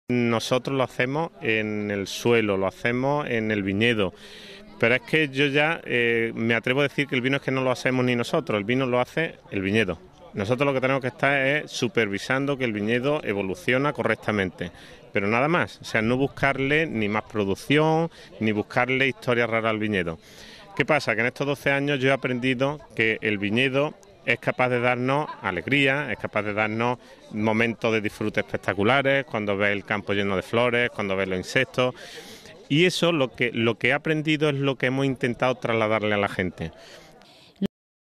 Francisco Robles explica la filosofía del programa "Sigue tu cepa" de Vinos Ecológicos Robles
El programa "Sigue tu cepa" trata de explicarle a la gente lo que hemos aprendido a lo largo de 12 años de viticultura ecológica: "El vino lo hacemos en el suelo, en el viñedo. Ya me atrevo a decir que el vino no lo hacemos ni nosotros, el vino lo hace el viñedo. Nosotros tenemos que estar supervisando que el viñedo evoluciona correctamente. Pero nada más".